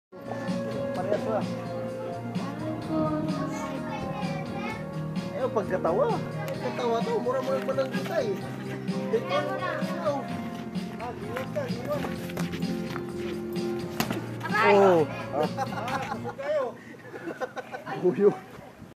One punch knockout
An epic battle between Filipino street fighters .😂😃😅